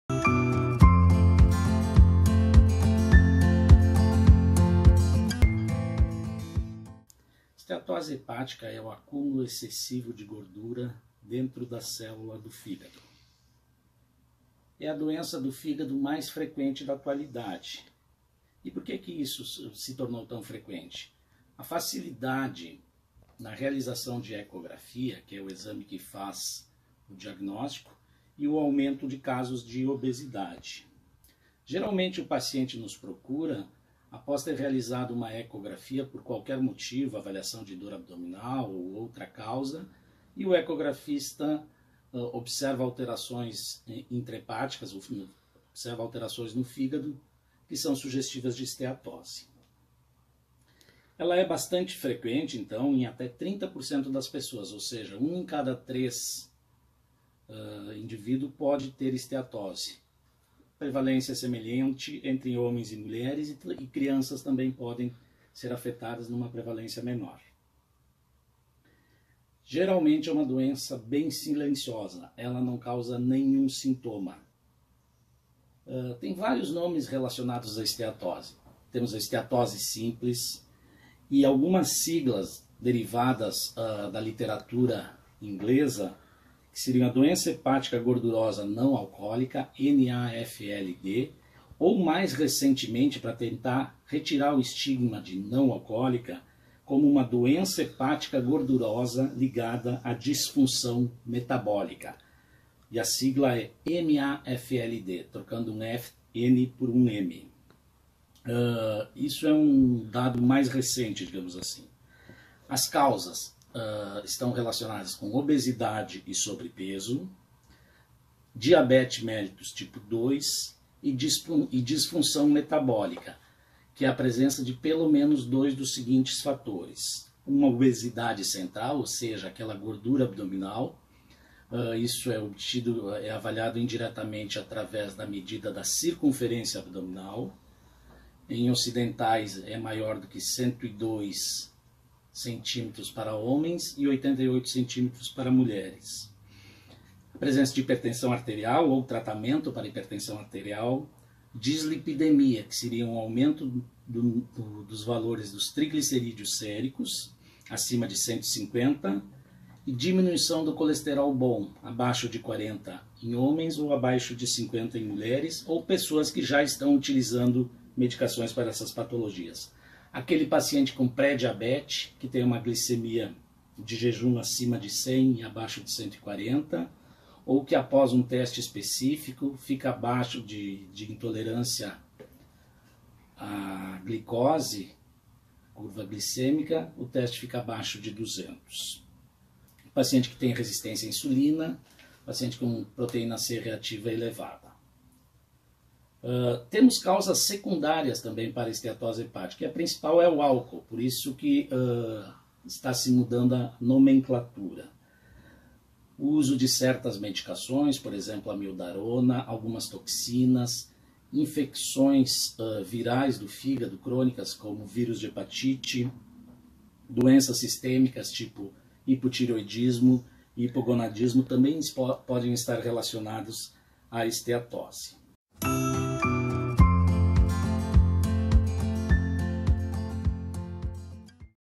Gordura no Fígado
O Dr Ilton Stella explica neste primeiro vídeo sobre esteatose hepática o que é a doença, causas e incidência. Saiba mais no iad.com.br. Outras informações pelo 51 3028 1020.